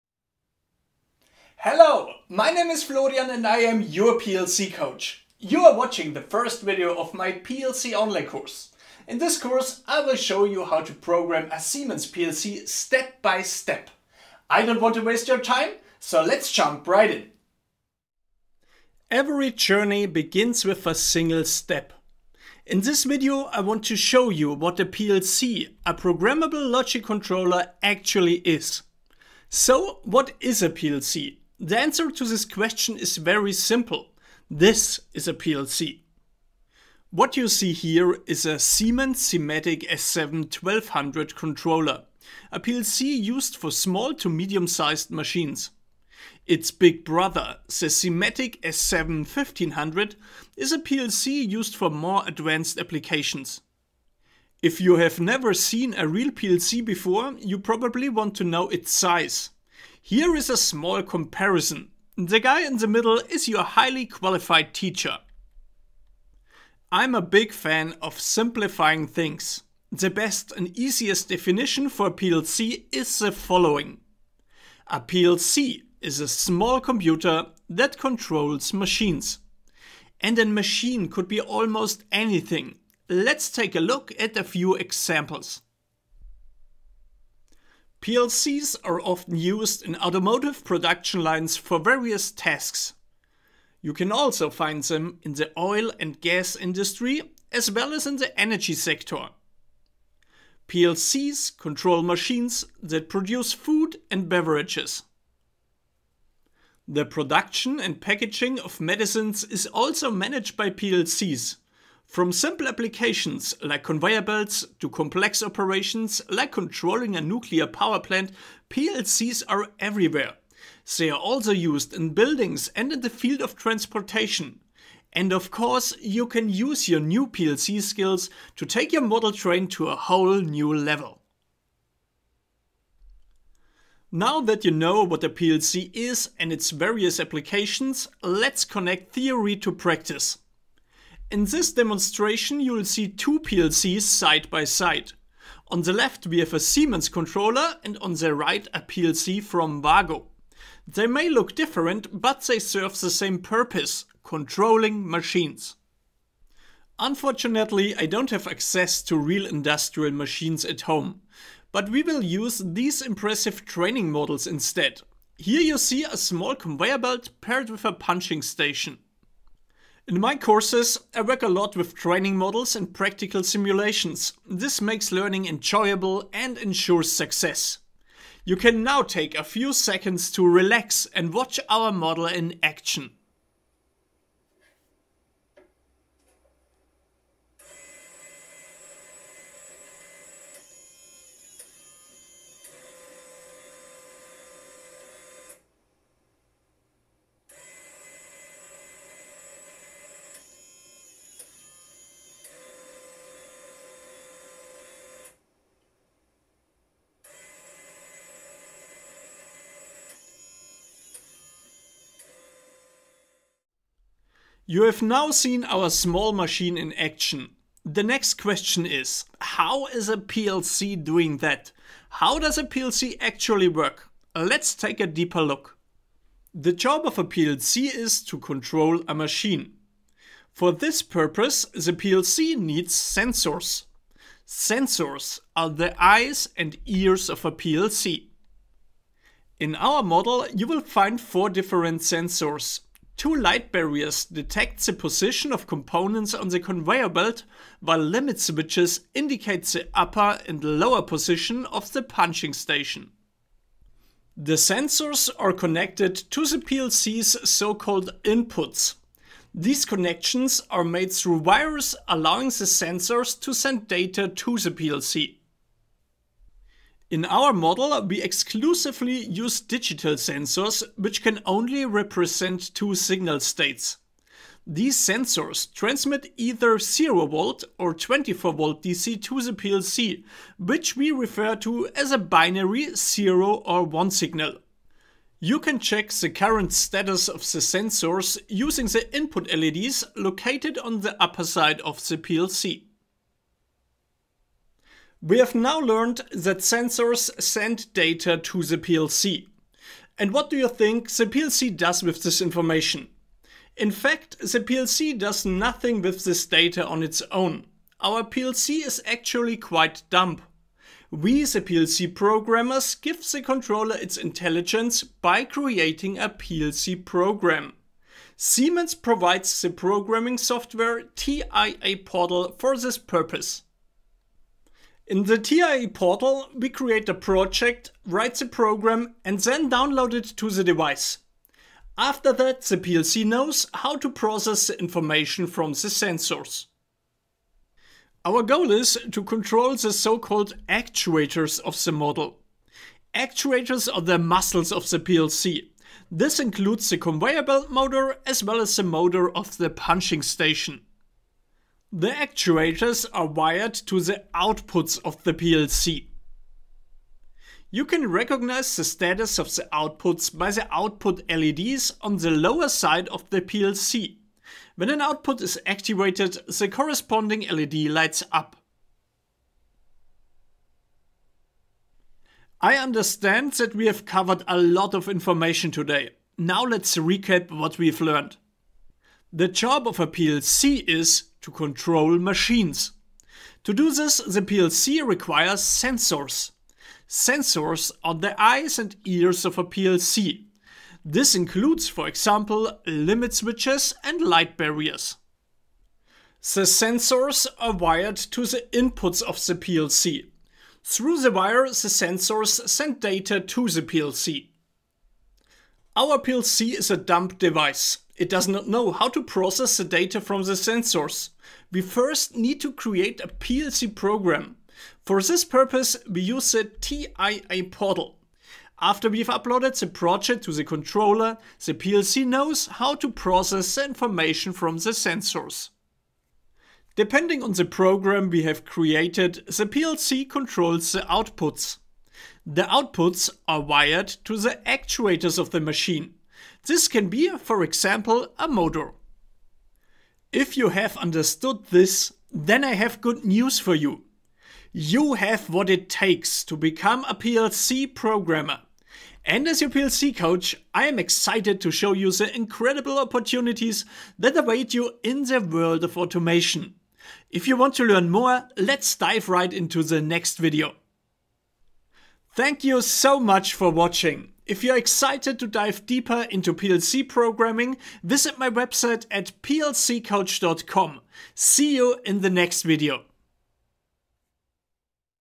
PLC Basic Course 1.1 | What is a PLC? | Learn PLC Programming in TIA Portal
What is a PLC? How does a PLC work? What is a PLC used for?
In the first chapter of my PLC Basic course, we will answer all of these questions.

My name is Florian and i am your PLC Coach. Together we will programm a Siemens Simatic Controller using the software TIA Portal.

▶ If you are interested in more content, please take a look at my homepage.

⌚Timestamps:
00:00 - Intro
00:18 - What is a PLC?
01:26 - Where to find PLCs
02:13 - Practical example
03:33 - How does a PLC work?
06:11 - Summary
07:19 - You have what it takes to become a PLC Programmer!
07:42 - Outro